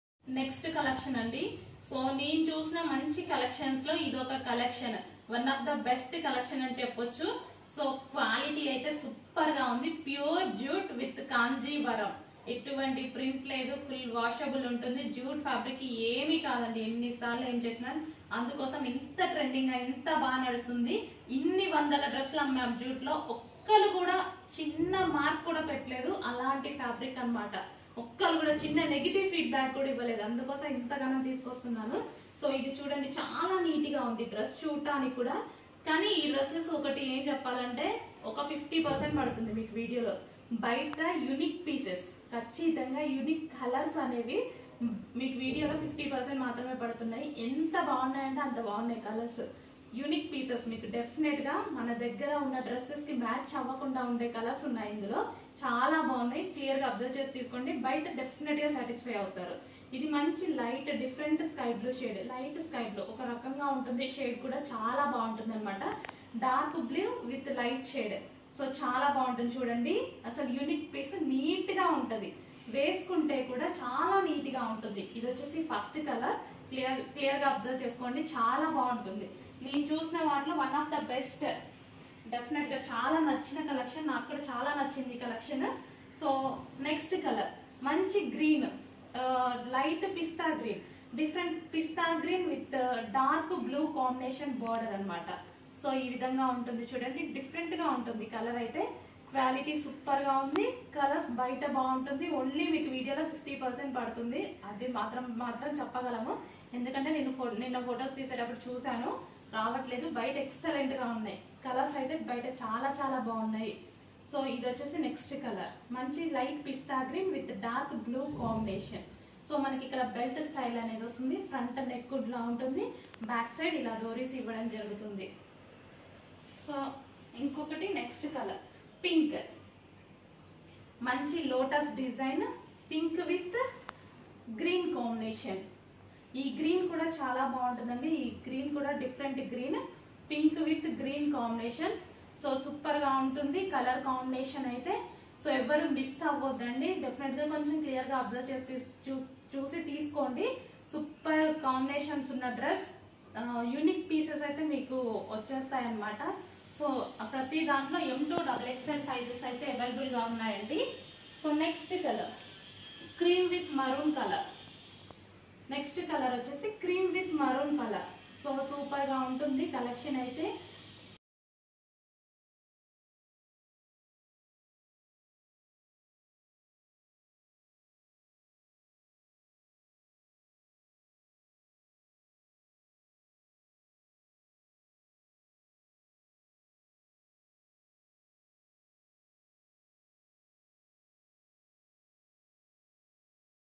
milana creation ❤️🌹 tq for watching vedio like and share subscribe my channel 🌹❤️👍🔔
Matrika presents

Customized long frocks

Famous jute with kanjivaram pattu borders🥳🥳 Full gera

With full  lining

Sizes  ,m 38 ,l 40 ,xl 42 , xxl 44

Length 56 to 57

Price 1400 ship extra

AP ts 60 ship

Other state 100

Don't miss full stock ready to dispatch